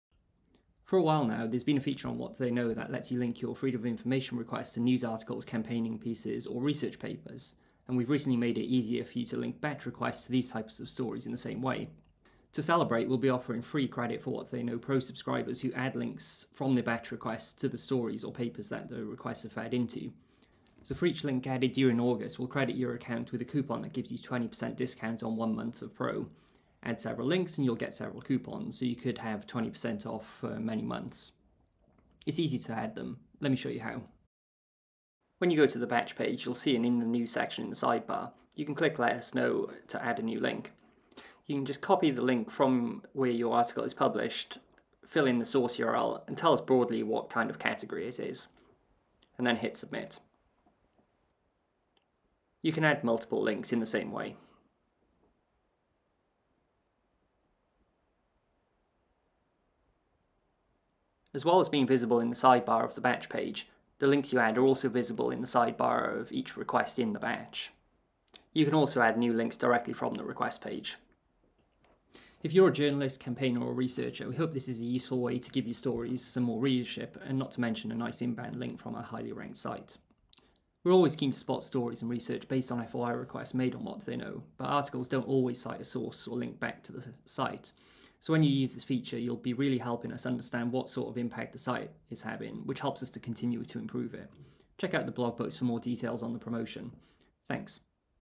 Tell us how you’ve used batch requests — get free Pro credit
Gareth explains how to add citations to batch FOI requests - and get a discount on your WhatDoTheyKnow Pro subscription for each one.

Check out WhatDoTheyKnow Pro, the tool for managing large numbers of Freedom of Information requests,

WhatDoTheyKnow Pro is a great resource for data and investigative journalists, researchers, campaigners and others who send multiple Freedom of Information requests and need to keep the process and data organised.

In this video, Gareth explains that you can get a reduced subscription if you add in links to the news stories, campaigns or research papers that result from your use of the tool.